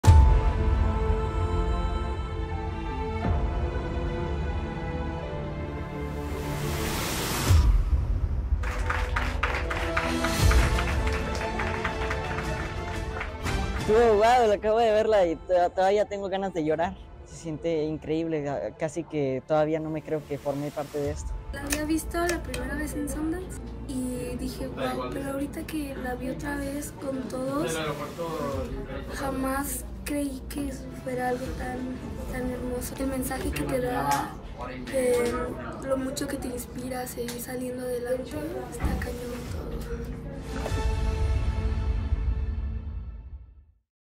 Emotiva reacción de los niños que actuaron en Radical al ver la película
Jennifer Trejo y Danilo Guardiola vieron Radical, película en la que actúan junto a Eugenio Derbez, y sus reacciones y las de su familia fueron muy inspiradoras.